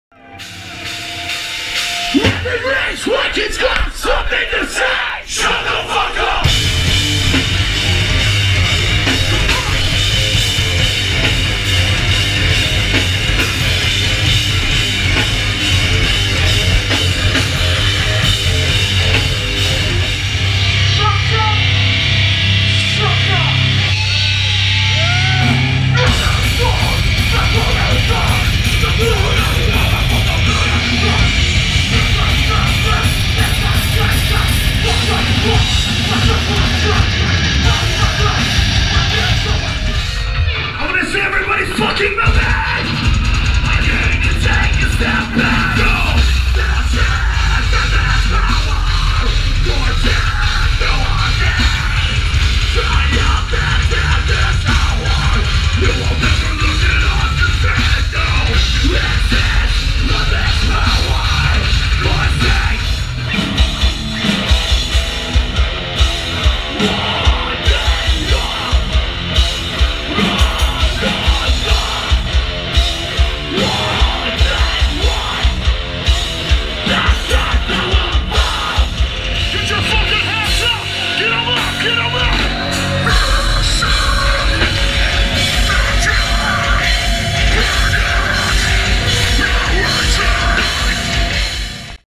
Ghostfest - Leeds - 5.9.15 - GoPro Clips of Stray From the Path, Oceano, Despised Icon and Nasty.
GoPro Hero 4 Black - 1080 superview with night vision and fish eye effect.